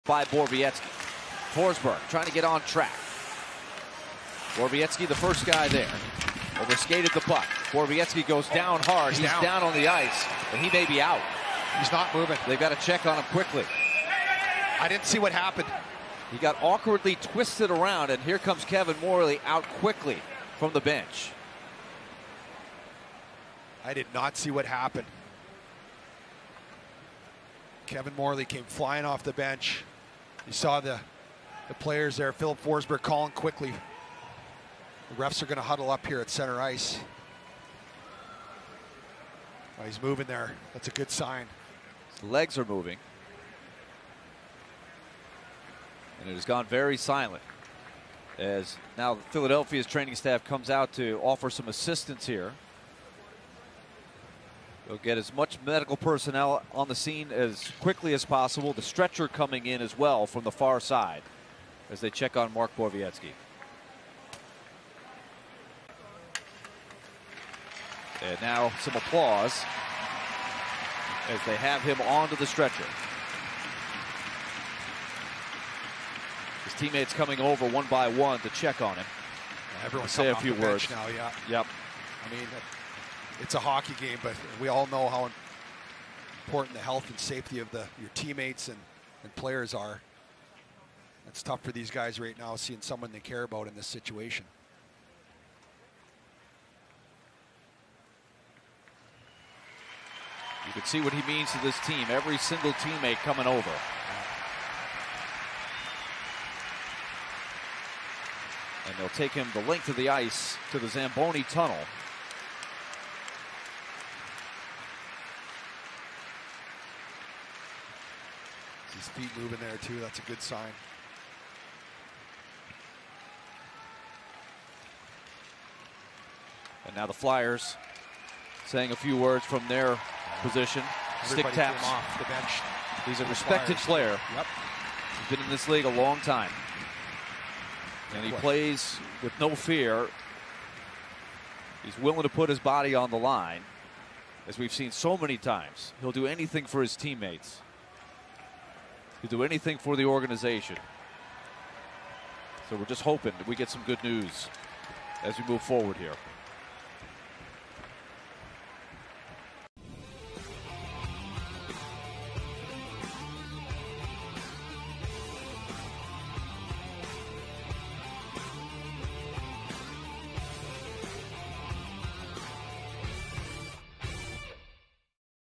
Mark Borowiecki Stretchered Off Vs. Flyers After Awkward Collision Into Boards
Nashville Predators' Mark Borowiecki is stretchered off the ice during the second period after an awkward collision with Philadelphia Flyers' Morgan Frost.